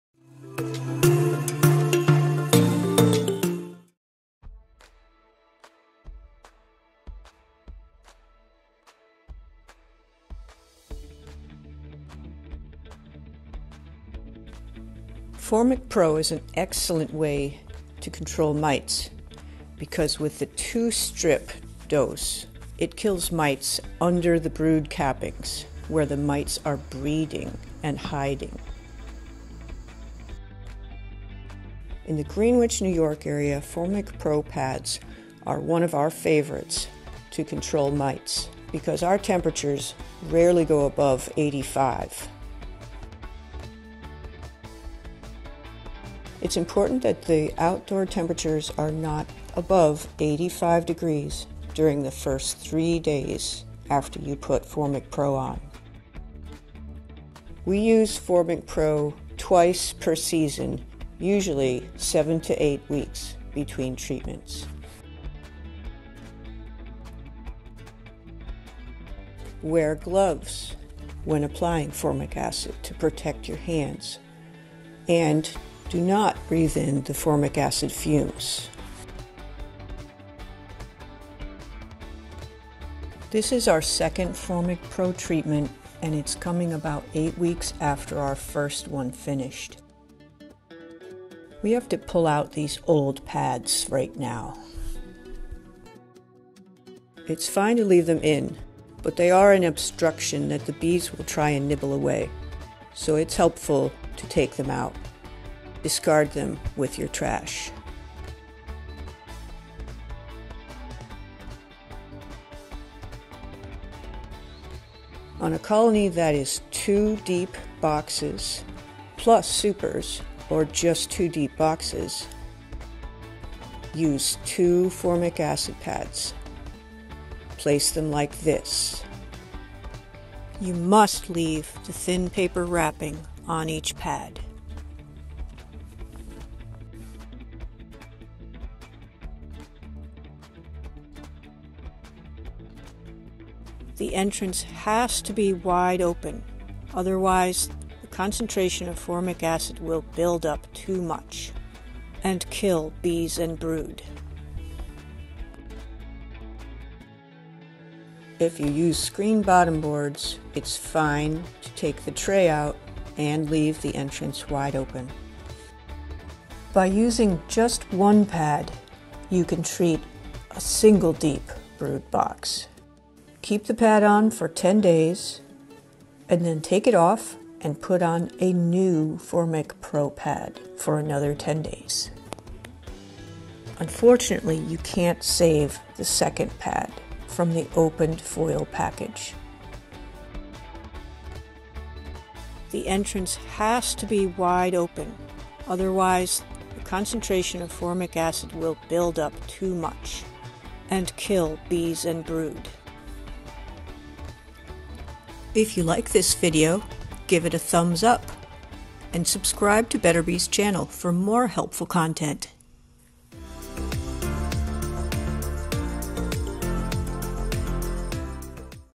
How to Use Formic Pro to Fight Varroa Mites in Honey Bees
Formic Pro is a great choice to control varroa mites in honey bee colonies if the average daily temperature in your area is 50 to 85 degrees Fahrenheit.

Formic Pro is the only miticide on the market that kills varroa where the mites breed: under the brood cappings. It can be used while your honey supers are on.